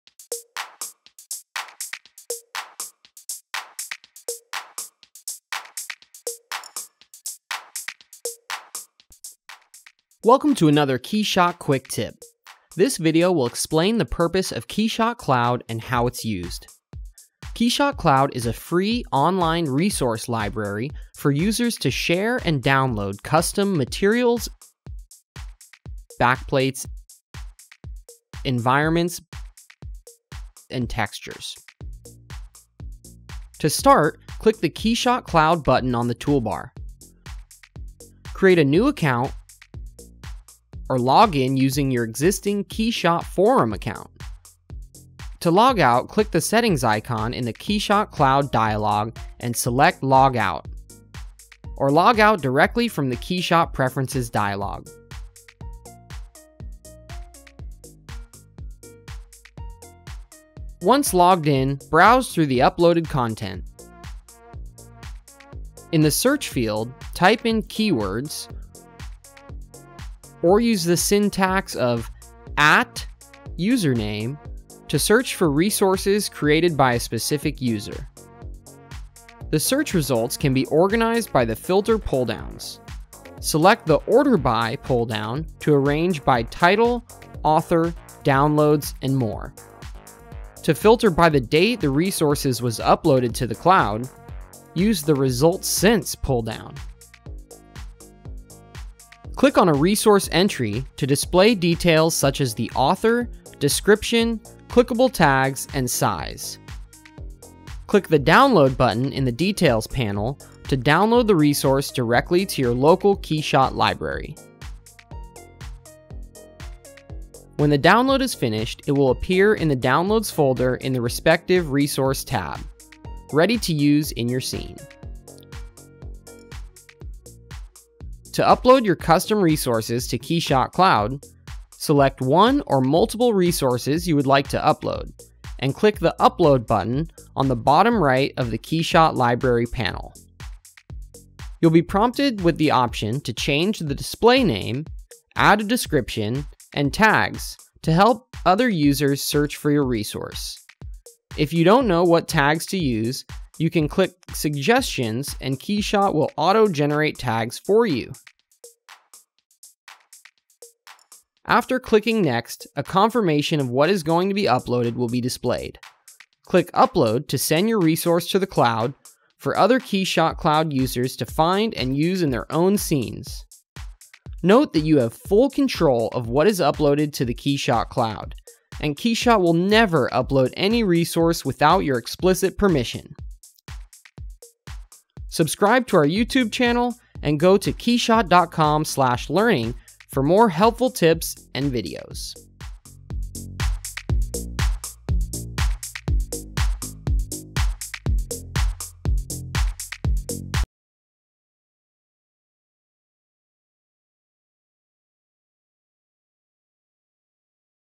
KeyShot Quick Tip: KeyShot Cloud
(level: Beginner) Use the KeyShot Cloud to find and share KeyShot materials, HDRI's, backplates, and textures. In this video, we will show you how to get started using the cloud to find and share resources with other KeyShot users.